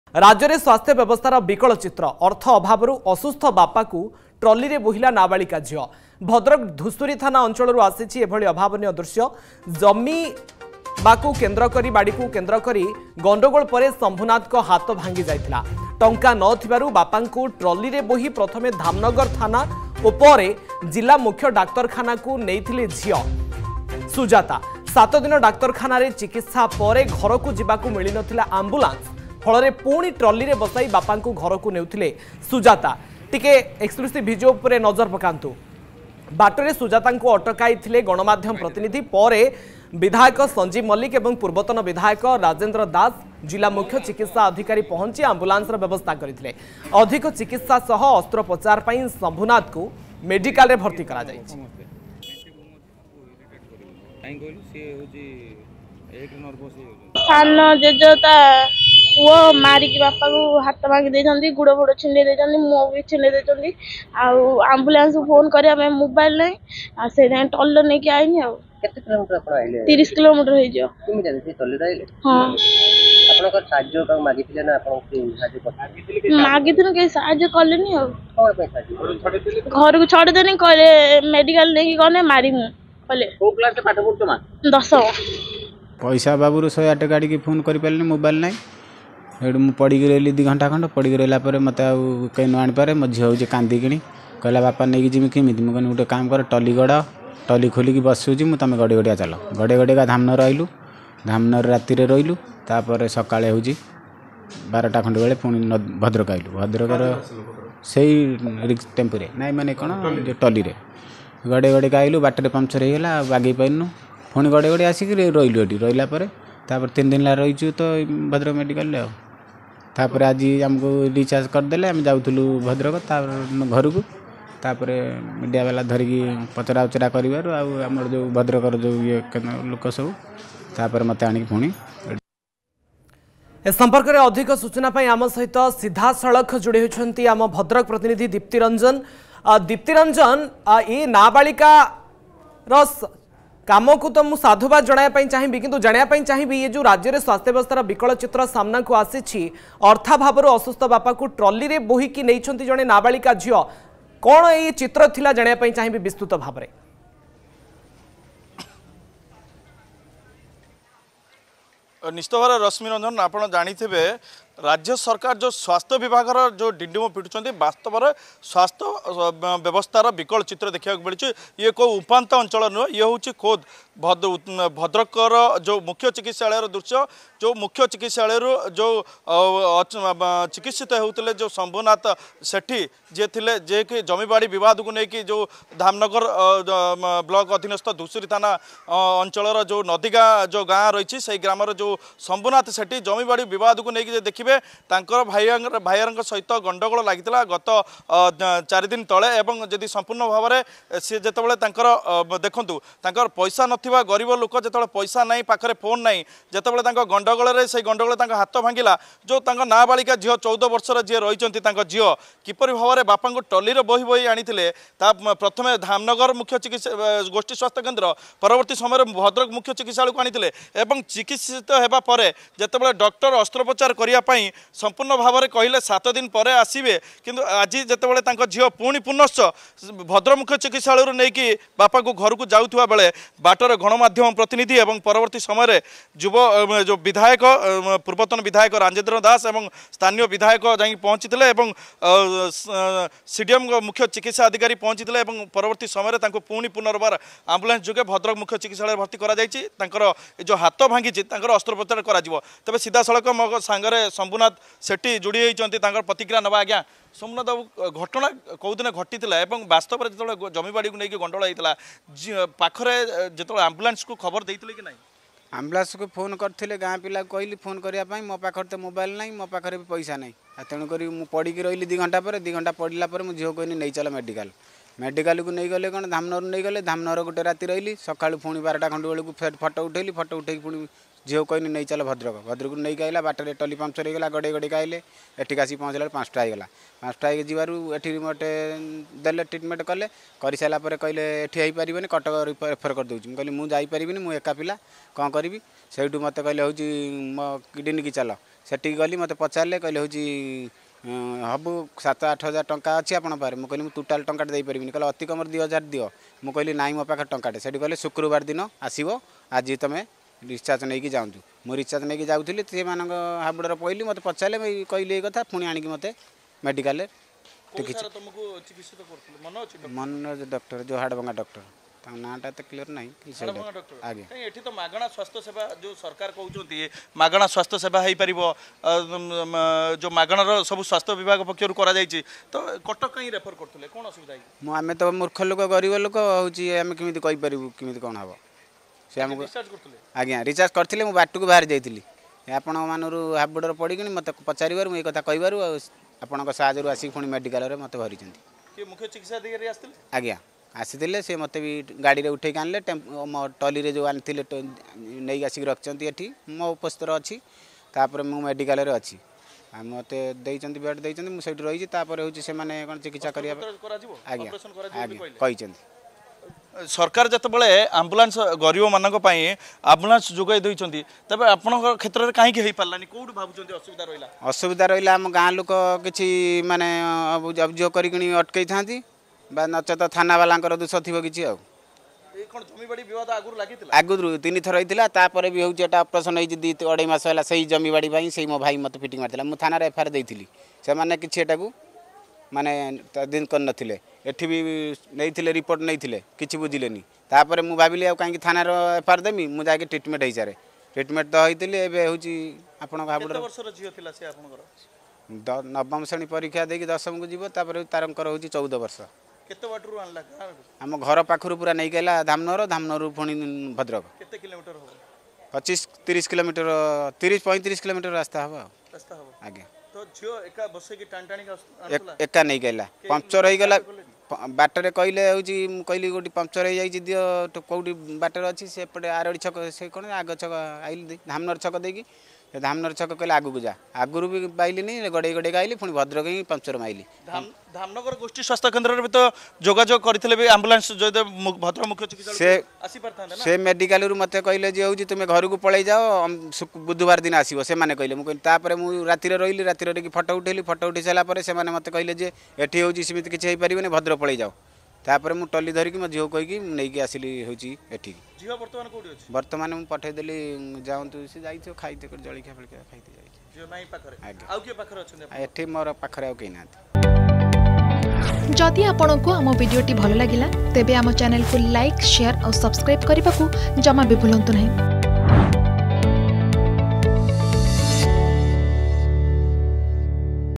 ଅର୍ଥ ଅଭାବରୁ ଅସୁସ୍ଥ ବାପାକୁ ଟ୍ରଲିରେ ବୋହିଲା ନାବାଳିକା ଝିଅ || Health Problem || Bhadrak
ରାଜ୍ୟରେ ସ୍ବାସ୍ଥ୍ୟ ବ୍ୟବସ୍ଥାର ବିକଳ ଚିତ୍ର । ଅର୍ଥ ଅଭାବରୁ ଅସୁସ୍ଥ ବାପାକୁ ଟ୍ରଲିରେ ବୋହିଲା ନାବାଳିକା ଝିଅ । ଭଦ୍ରକ ଧୂଷୁରୀ ଥାନା ଅଞ୍ଚଳରୁ ଆସିଛି ଏଭଳି ଅଭାବନୀୟ ଦୃଶ୍ୟ ।